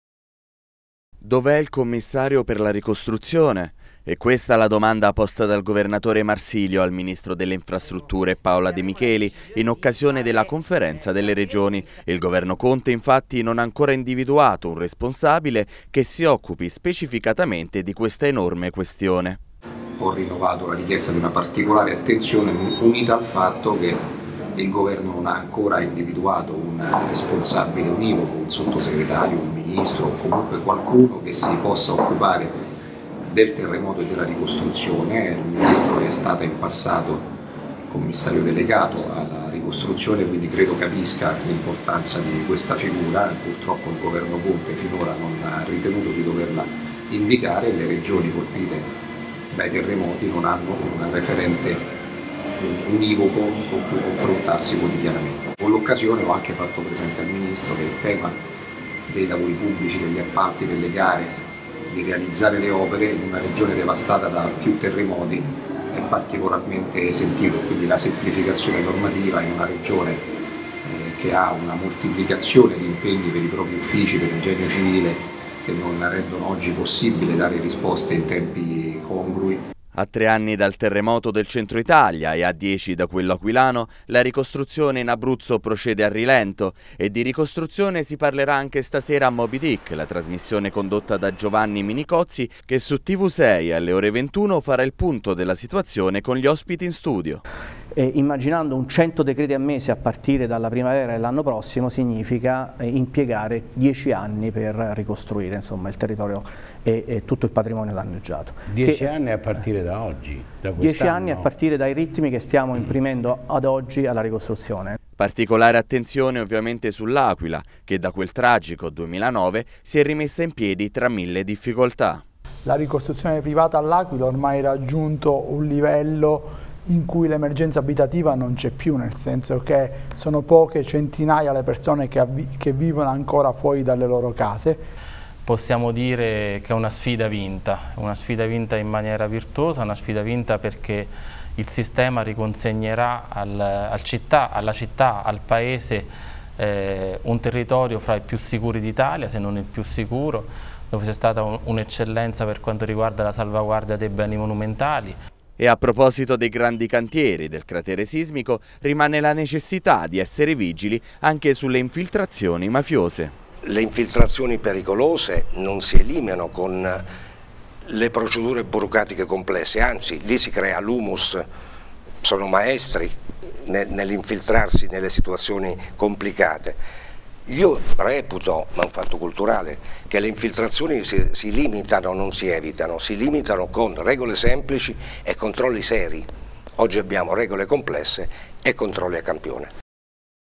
Ricostruzione. Marsilio al Governo: "Subito il Commissario"
Il presidente Marco Marsilio ha partecipato insieme agli altri governatori alla Conferenza delle regioni. Presente il ministro delle infrastrutture De Micheli, alla quale il governatore abruzzese ha richiesto la nomina del responsabile della ricostruzione. E di ricostruzione si parlerà anche stasera alle 21 a Mob Dick.